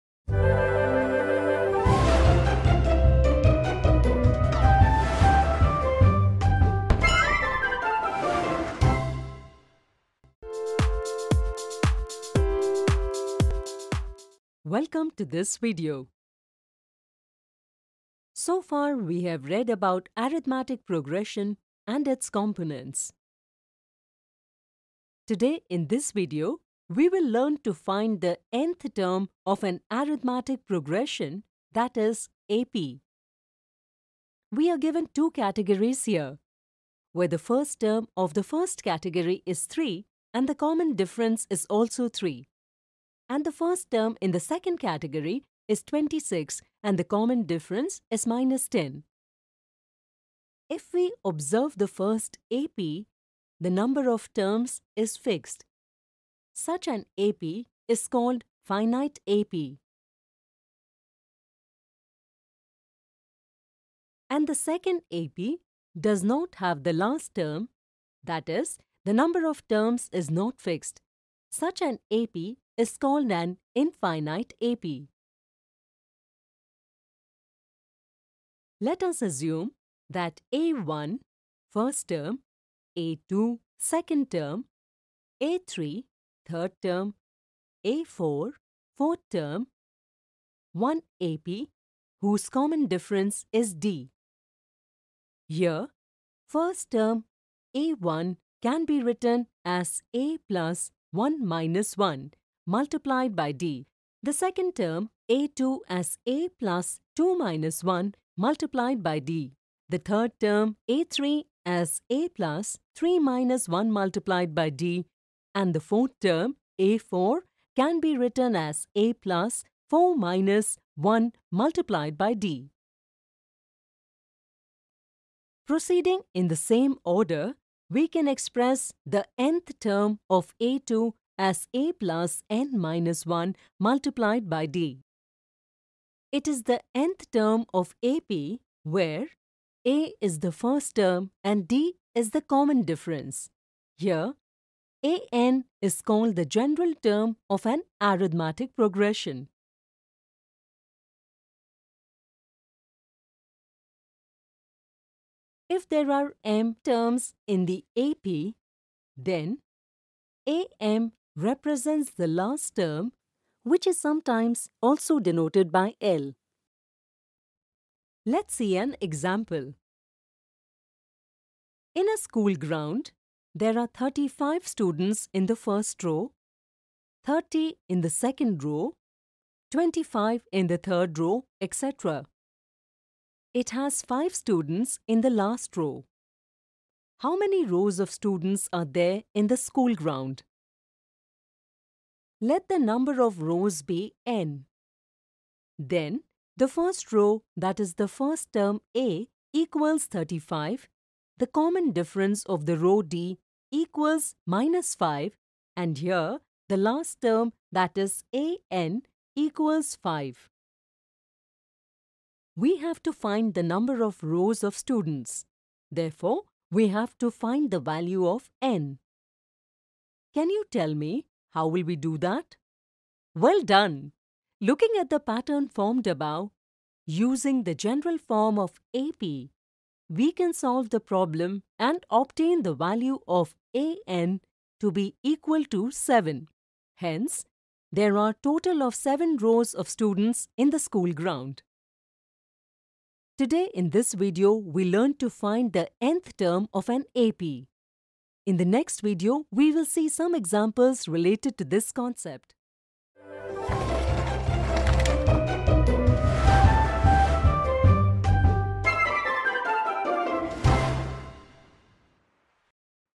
nth term of an Arithmetic Progression (AP) | Part 1/3 | English | Class 10
nth term of an Arithmetic Progression (AP) | Part 1/3 | English | Class 10 | NCERT

nth Term of an Arithmetic Progression | Quadratic Equations | Grade 10 | NCERT | Arithmetic Progressions (3)

Welcome to part 1 of nth Term of an Arithmetic Progression. In this video, we will discuss the concept of nth Term of an Arithmetic Progression.